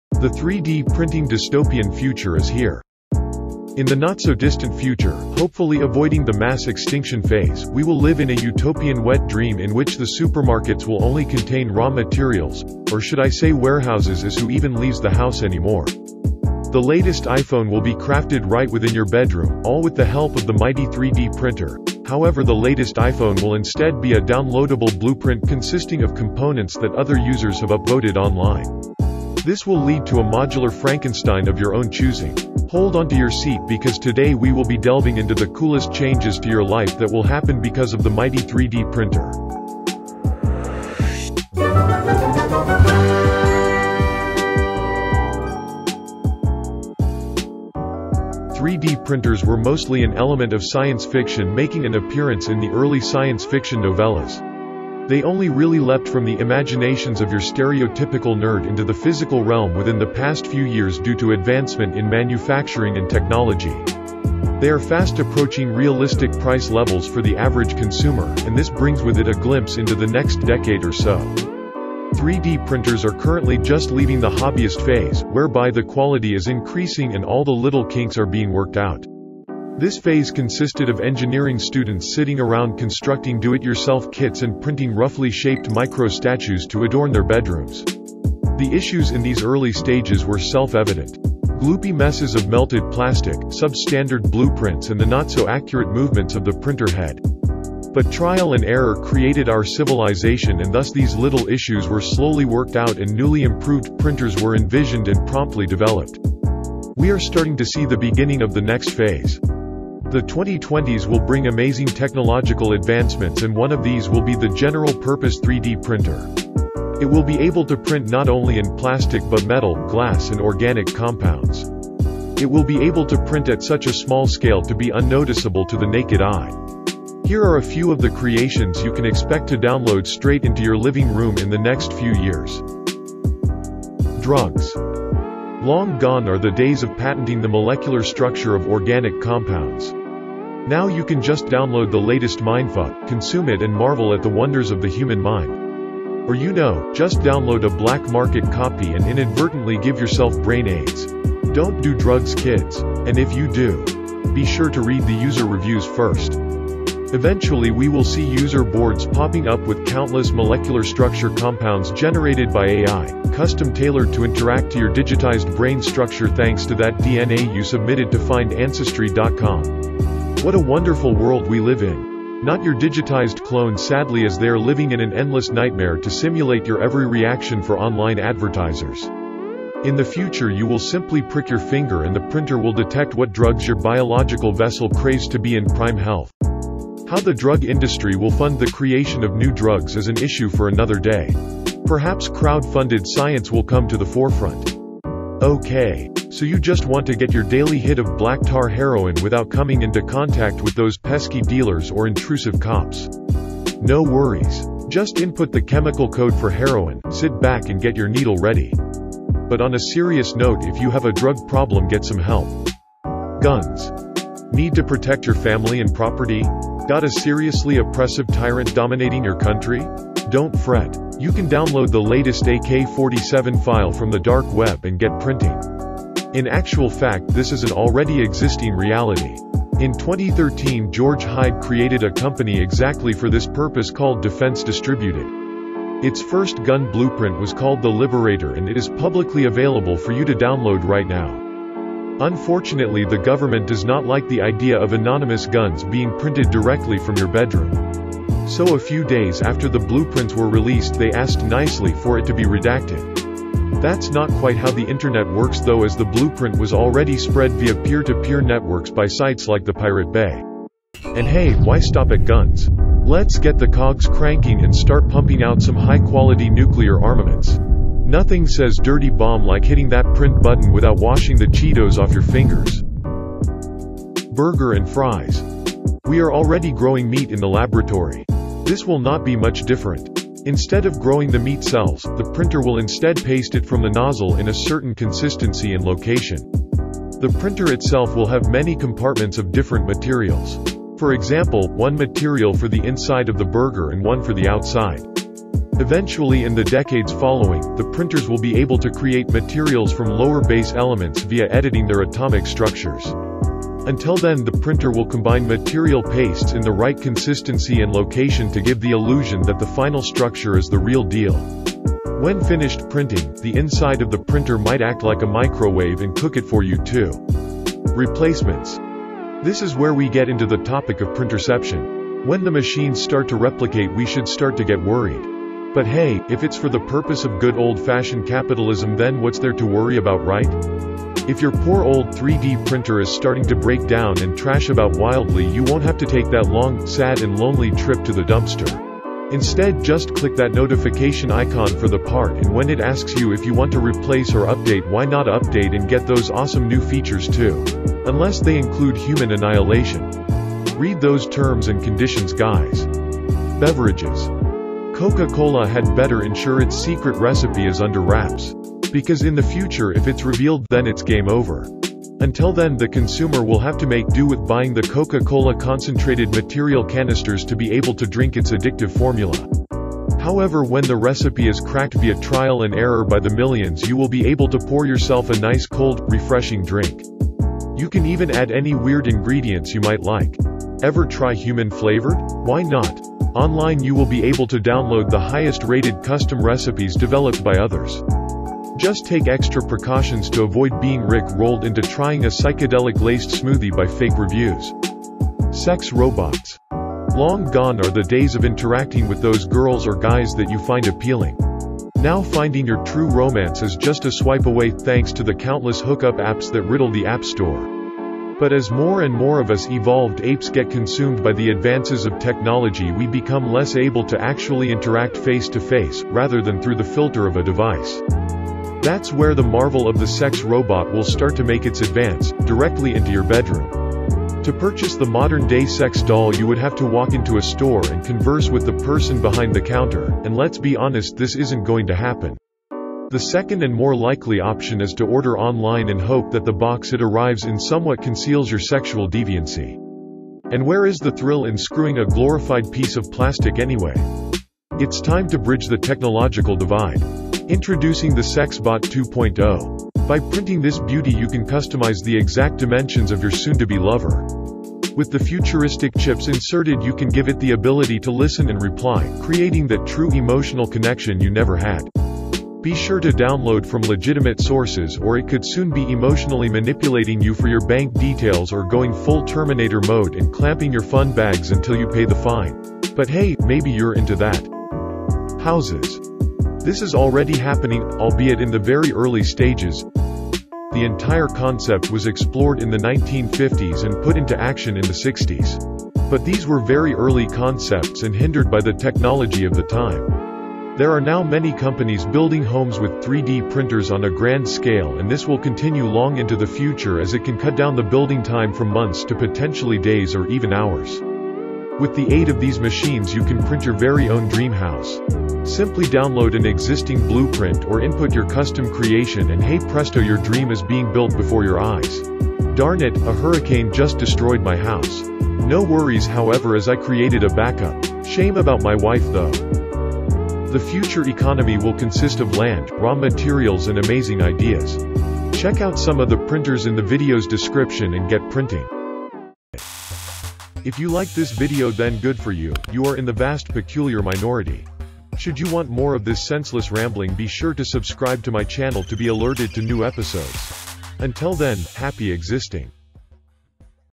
The 3D Printing Dystopian Future Is Here
3D printing will bring the next stage of consumer evolution. We will gain access to a near unlimited range of products that we can customize and mix to fit our lives perfectly. Let me know down in the comment what you think we will be printing in the next few years?

Comgrow Creality Ender 3 Pro 3D Printer

FlashForge 3D Printer Creator Pro